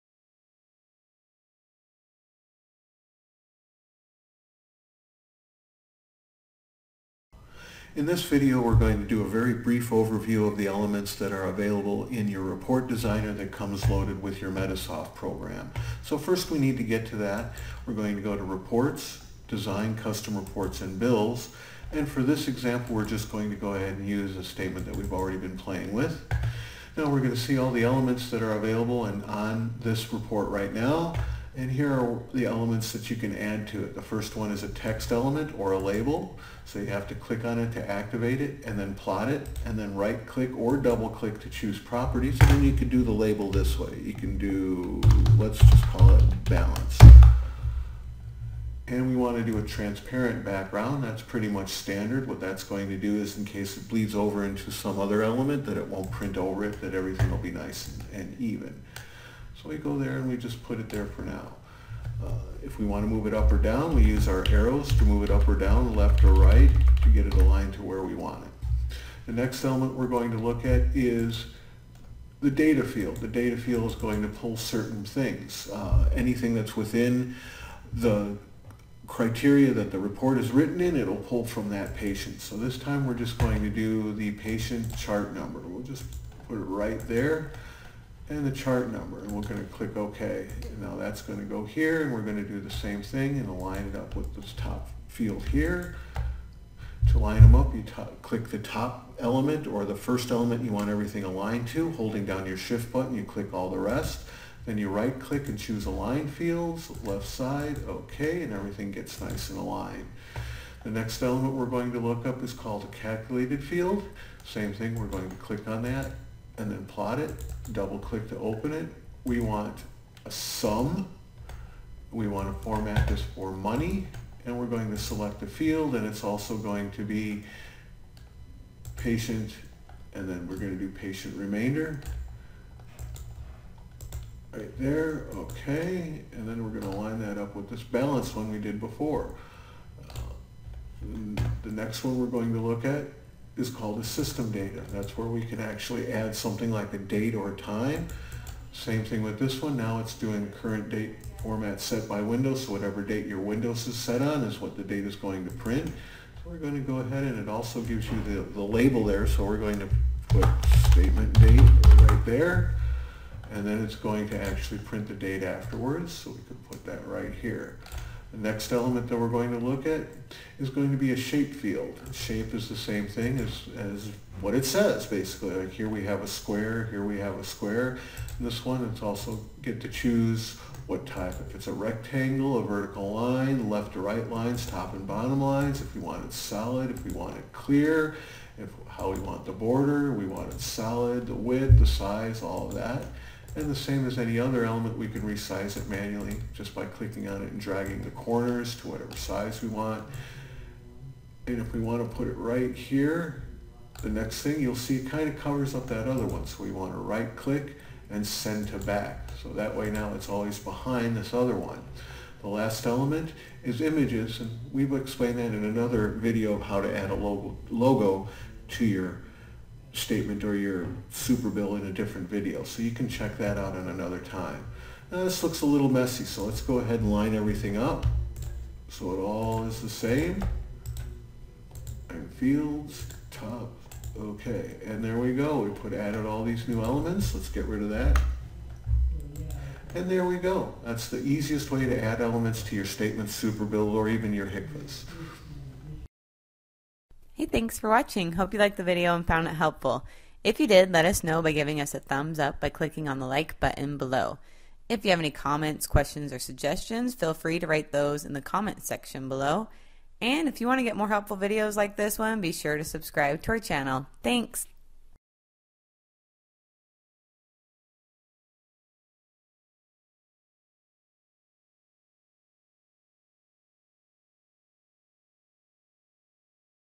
Medisoft Custom Reports Designer Overview
In this awesome video - learn how to customize your statements, superbills or other reports.  This video also refers to another video we previously made

Video Transcript:
In this video we're going to do a very brief overview of the elements that are available in your report designer that comes loaded with your Medisoft program. First we need to get to that. We're going to go reports, design custom reports and bills, and for this example we're just going to go ahead and use a statement that we've already been playing with. Now we're going to see all the elements that are available, and on this report right now. Here are the elements that you can add to it.
 The first one is a text element or a label, so you have to click on it to activate it, and then plot it, and then right click or double click to choose properties. Then you could do the label this way, you can do, let's just call it balance. We want to do a transparent background, that's pretty much standard. What that's going to do is in case it bleeds over into some other element that it won't print over it, that everything will be nice and even. We go there, and we just put it there for now. If we want to move it up or down we use our arrows to move it up or down, left or right, to get it aligned to where we want it.
 The next element we're going to look at is the data field. The data field is going to pull certain things. Anything that's within the criteria that the report is written, in it'll pull from that patient. This time we're just going to do the patient chart number. We'll just put it right there, and the chart number, and we're going to click OK. Now that's going to go here, and we're going to do the same thing, and align it up with this top field here. To align them up you click the top element, or the first element you want everything aligned to, holding down your shift button you click all the rest. Then you right click and choose align fields, left side, OK, and everything gets nice and aligned.
 The next element we're going to look up is called a calculated field. Same thing, we're going to click on that, and then plot it. Double click to open it. We want a sum, we want to format this for money, and we're going to select a field, and it's also going to be patient, and then we're going to do patient remainder right there, OK. Then we're going to align that up with this balance one we did before.
 The next one we're going to look at is called the system data. That's where we can actually add something like a date or time. Same thing with this one, now it's doing current date format set by Windows, so whatever date your Windows is set on is what the date is going to print. We're going to go ahead, and it also gives you the label there, so we're going to put statement date right there, and then it's going to actually print the date afterwards, so we can put that right here.
 The next element that we're going to look at is going to be a shape field. Shape is the same thing as what it says basically. Here we have a square, here we have a square, and this one it's also get to choose what type if it's a rectangle, a vertical line, left to right lines, top and bottom lines. If we want it solid, if we want it clear, how we want the border. We want it solid, with the size all of that, and the same as any other element we could resize it manually, just by clicking on it and dragging the corners to whatever size we want. If we want to put it right here, the next thing you'll see it kind of covers up that other one, so we want to right click and send to back. That way now it's always behind this other one.
 The last element is images, and we will explain that in another video, of how to add a logo to your statement or your super bill in a different video, so you can check that out at another time. Now this looks a little messy. Let's go ahead and line everything up, so it all is the same, and fields, top, okay. There we go, we added all these new elements. Let's get rid of that, and there we go. That's the easiest way to add elements to your statement, super bill, or even your HCFA’s.